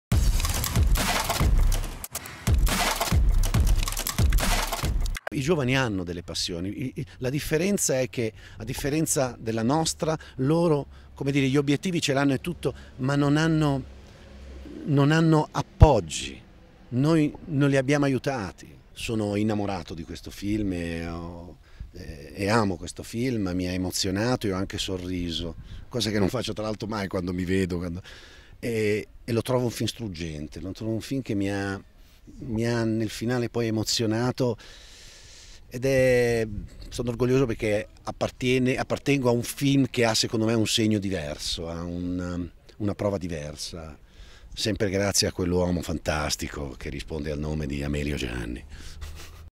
Mondofilm - Antonio Albanese
Intervista ad Antonio Albanese, chapliniano e grottesco protagonista del film di Gianni Amelio, "L'intrepido" in concorso al Lido.